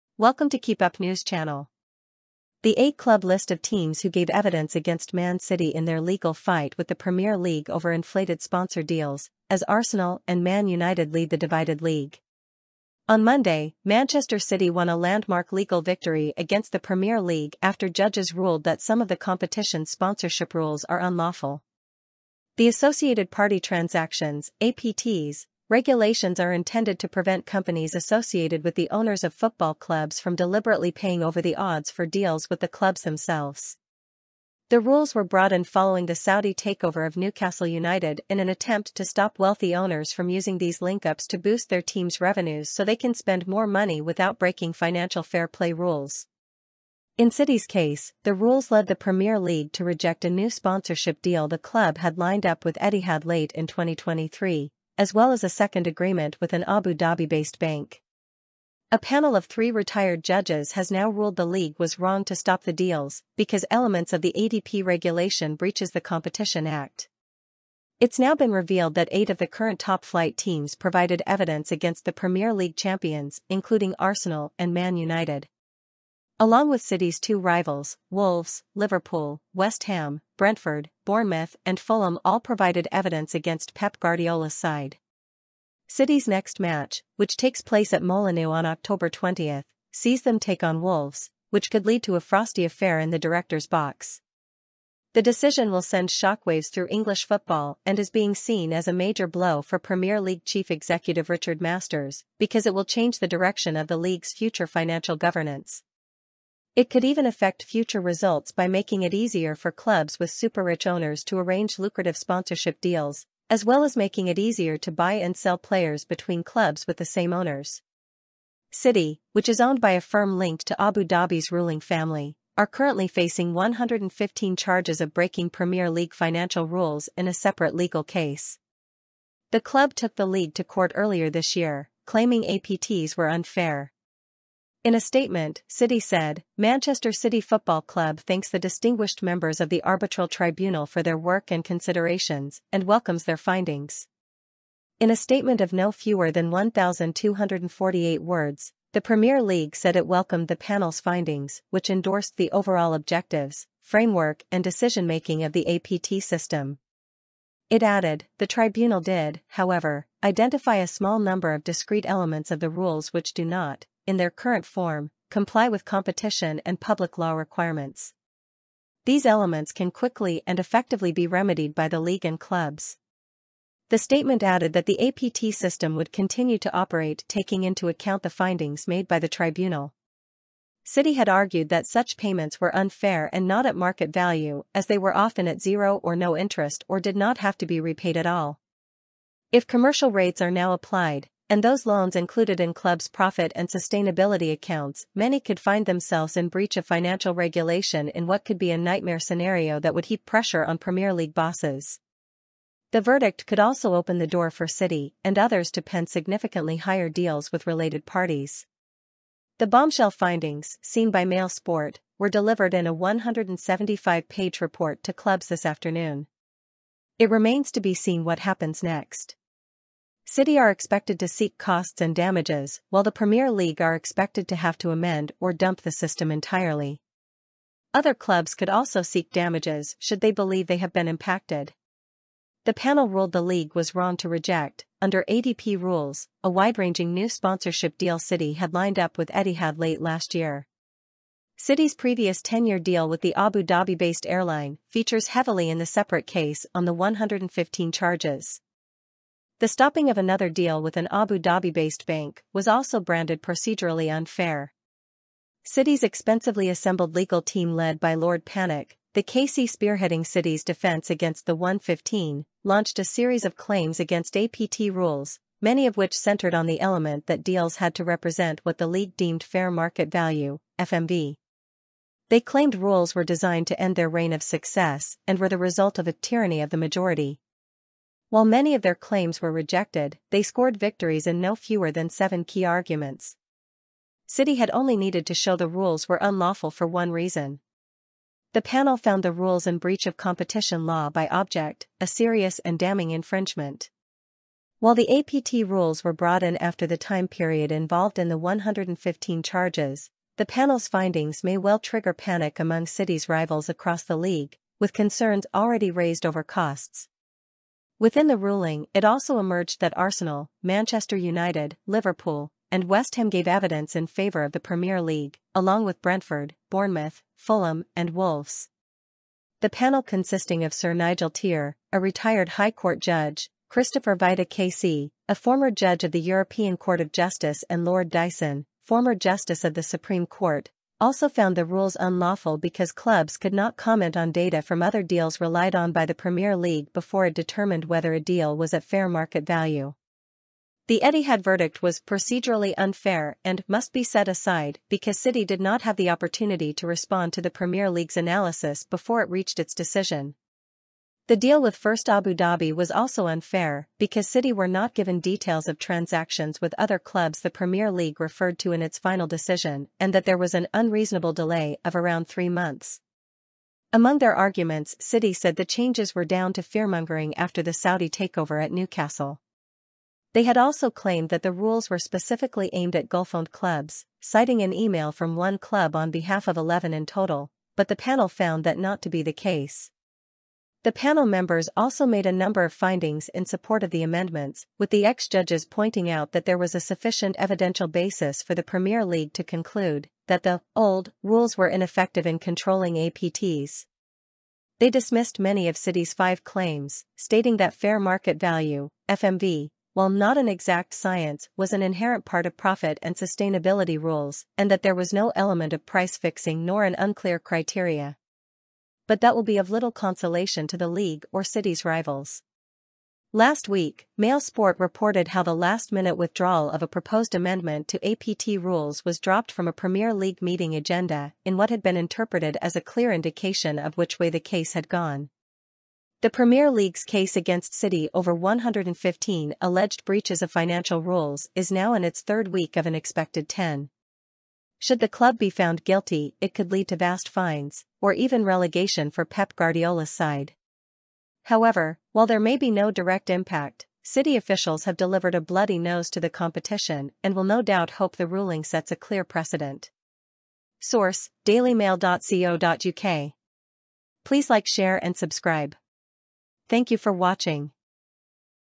Sport News: The eight-club list of teams who gave evidence AGAINST Man City in their legal fight...
Sport News: The eight-club list of teams who gave evidence AGAINST Man City in their legal fight with the Premier League over 'inflated' sponsor deals, as Arsenal and Man United lead the divided league